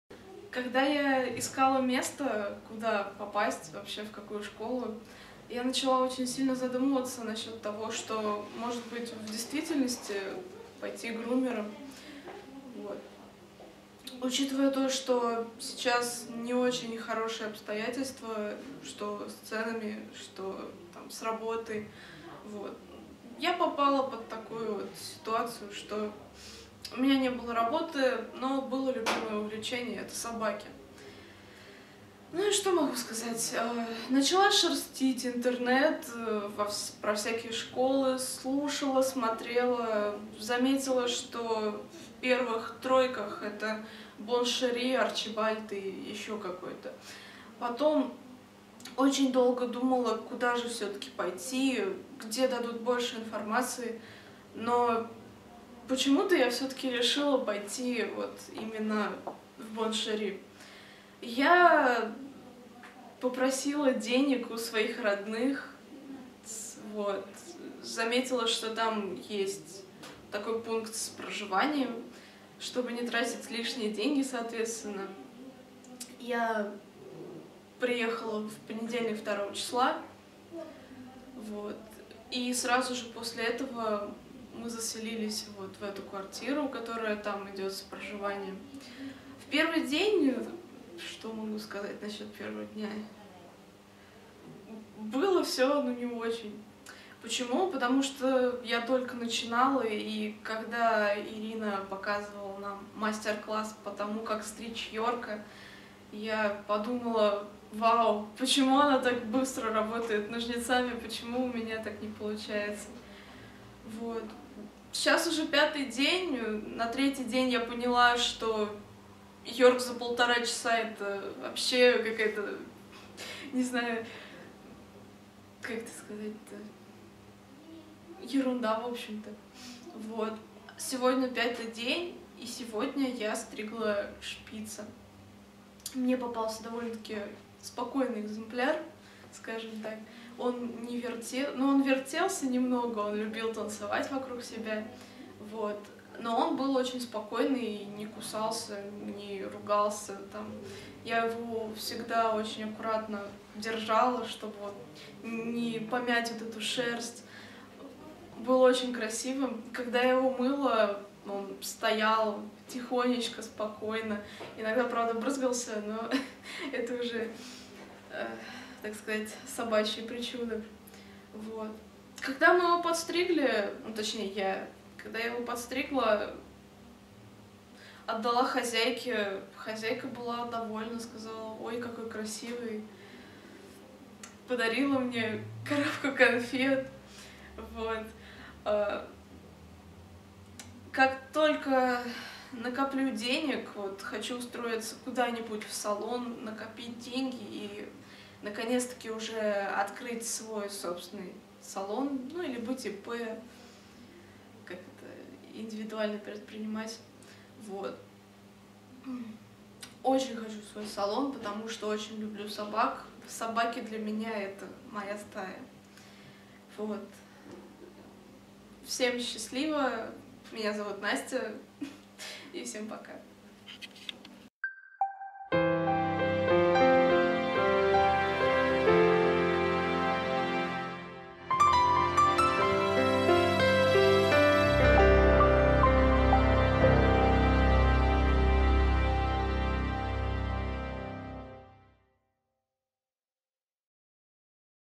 Жизнь, практика и мечты Насти из Раменского
Стоит смотреть:
- как нашла деньги на обучение
- как менялось отношение к грумингу
- какие теперь цели у Насти
- фрагменты практического обучения а Академии груминга Боншери.